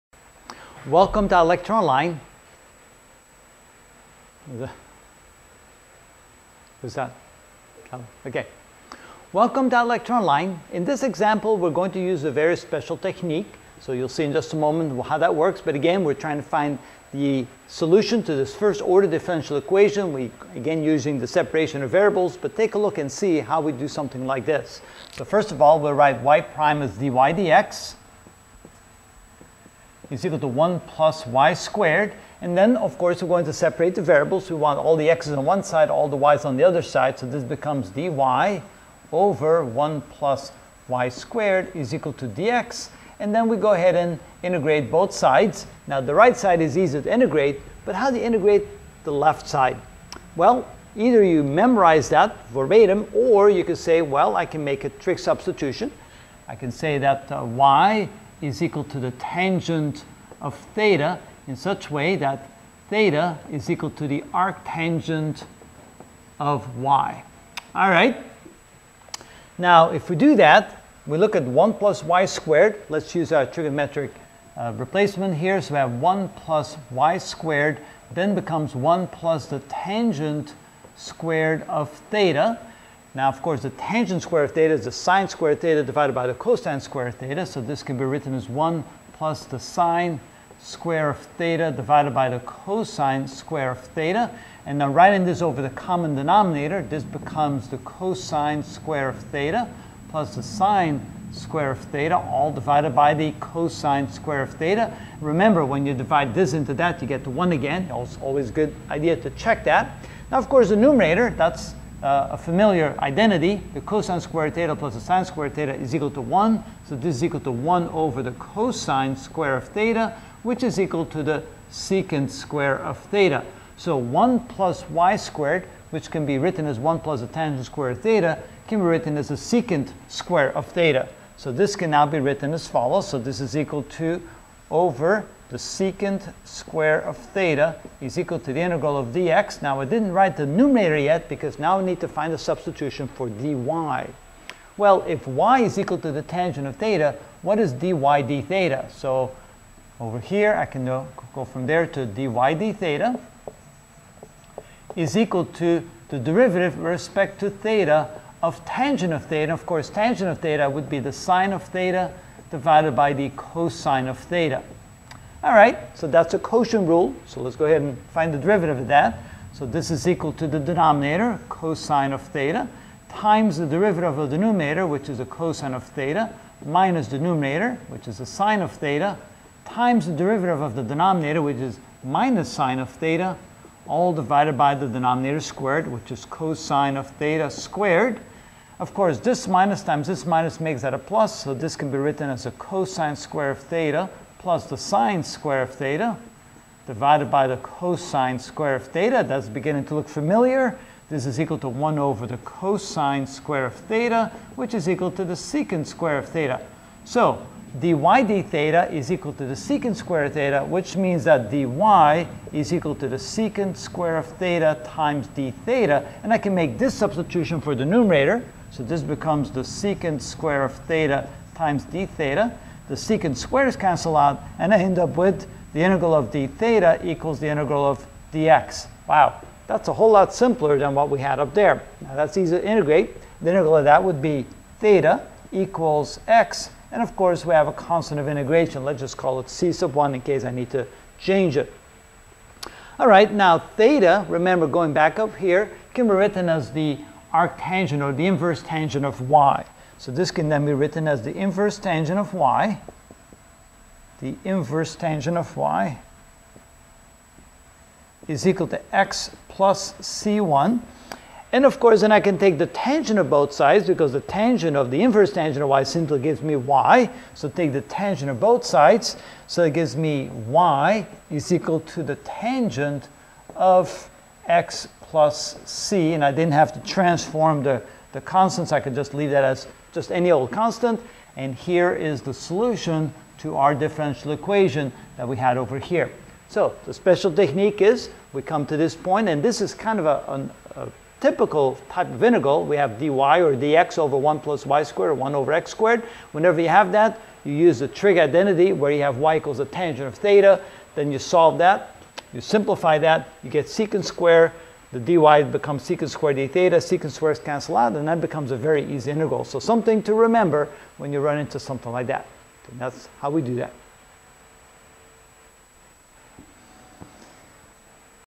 Differential Equation - 1st Order Solutions (3 of 8) Separation of Variables: Example 3
In this video I will find the 1st order solution to y'=1+y^2.